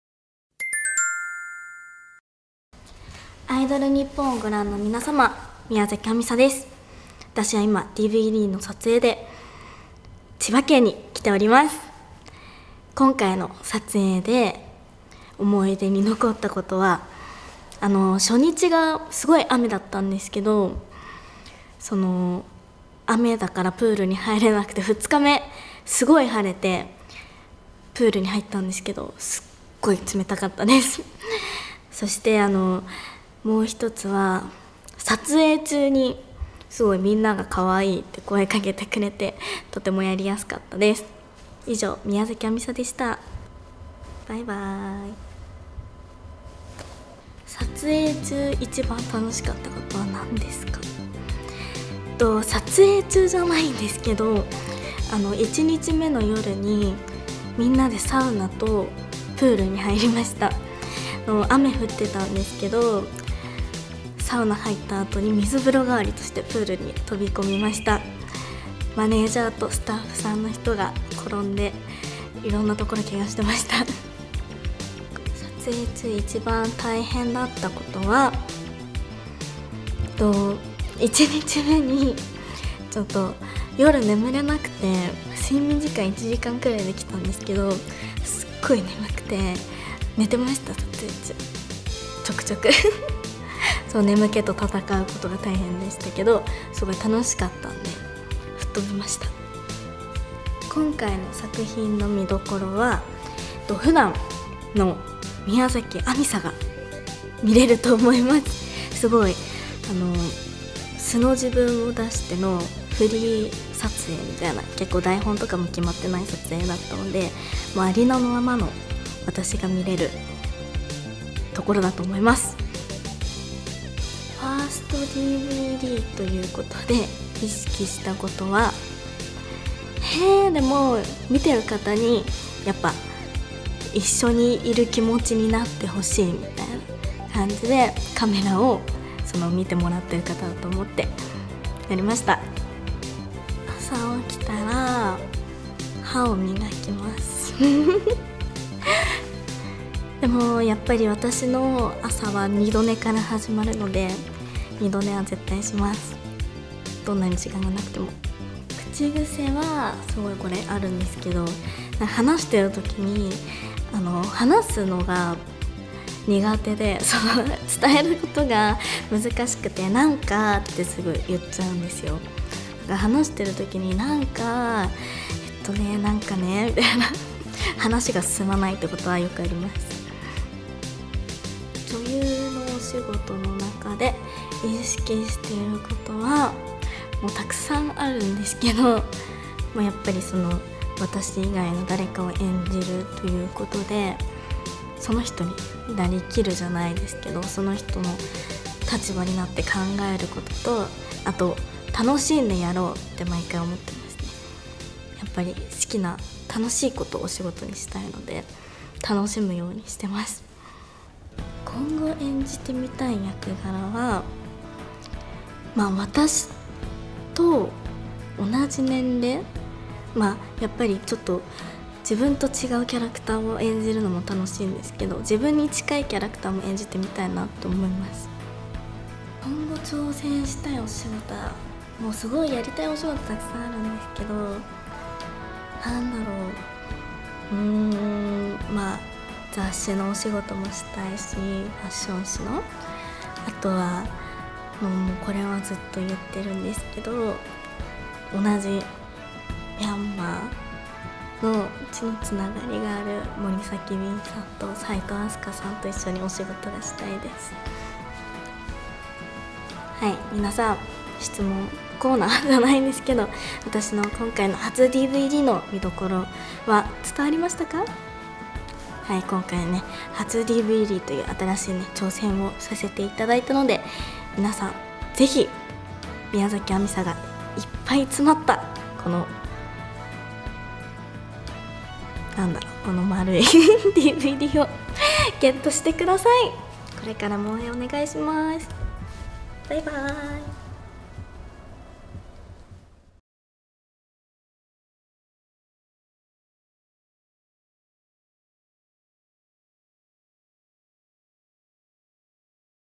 【宮崎あみさ】1stDVDコメント＆インタビュー
■宮崎あみさ「PURE」
これが、あみさの本当の姿！！

『暴太郎戦隊ドンブラザーズ』の悪役ヒロイン・ソノニとして活躍中の、宮崎あみさ1stDVD！！
透明感溢れるルックスと、時折見せる妖艶な雰囲気でアナタを恋に落とす。
まだ知らない彼女の本当の姿をご覧あれ…。

出演者：宮崎あみさ